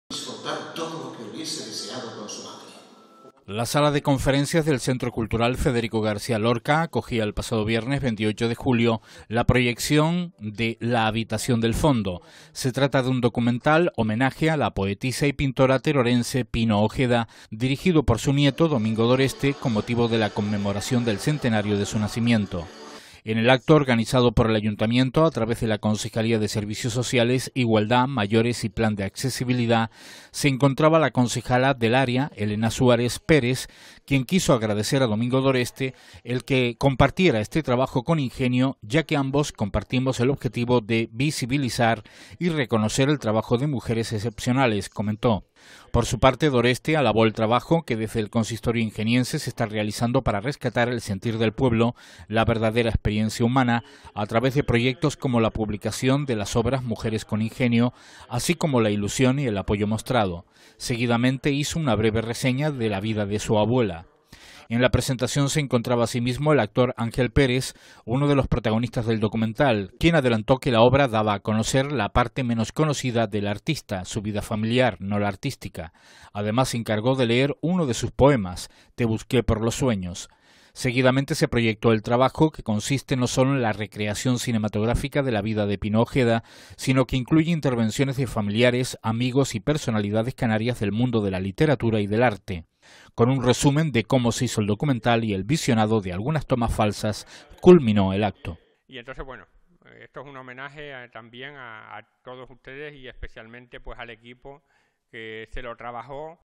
Ingenio se une al reconocimiento de la poetisa y pintora Pino Ojeda
Ingenio se une al reconocimiento de la poetisa y pintora Pino Ojeda en el centenario de su nacimiento

La Sala de Conferencias del Centro Cultural Federico García Lorca de la Villa de Ingenio acogía el pasado viernes, 28 de julio, la proyección de "La Habitación del Fondo". Se trata de un docudrama homenaje a la poetisa y pintora terorense Pino Ojeda dirigido por su nieto, Domingo Doreste, con motivo de la conmemoración del centenario de su nacimiento.

En el acto, organizado por el Ayuntamiento de Ingenio a través de la Concejalía de Servicios Sociales, Igualdad, Mayores y Plan de Accesibilidad, se encontraba la concejala del área, Elena Suárez Pérez, quien quiso agradecer a Domingo Doreste el que llevara este trabajo a Ingenio ya que ambos "compartimos el objetivo de visibilizar y reconocer el trabajo de mujeres excepcionales," comentó. Por su parte Doreste alabó el trabajo que desde el consistorio ingeniense se está realizando para rescatar "el sentir del pueblo, la verdadera experiencia humana" a través de proyectos como la publicación de las obras "Mujeres con Ingenio" así como la ilusión y el apoyo mostrado. Seguidamente hizo una breve reseña de la vida de su abuela.

En la presentación se encontraba asimismo el actor Ángel Pérez, uno de los protagonistas del documental, quien adelantó que la obra daba a conocer la parte menos conocida de la artista, su vida familiar no la artística. Además se encargó de leer uno de sus poemas, "Te busqué por los sueños".

Seguidamente se proyectó el trabajo, que consiste no sólo en la recreación cinematográfica de la vida de Pino Ojeda sino que incluye intervenciones de familiares, amigos y personalidades canarias del mundo de la Literatura y el Arte. Con un resumen de cómo se hizo el documental y el visionado de algunas tomas falsas culminó el acto.